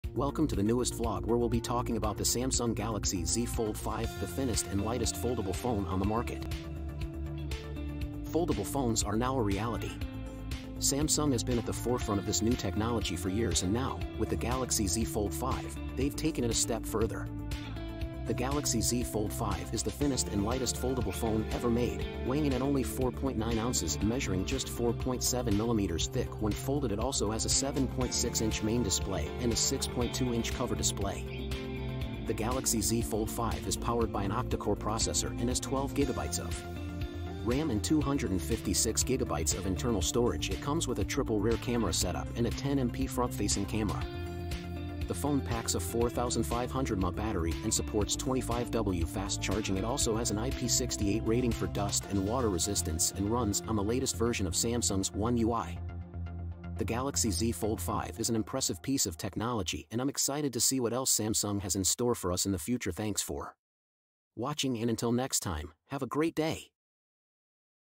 The Samsung Galaxy Z Fold 5 - The Thinnest and Lightest Foldable Phone
In this vlog, we take a look at the Samsung Galaxy Z Fold 5 - the thinnest and lightest foldable phone on the market. We'll talk about its specs, features, and more!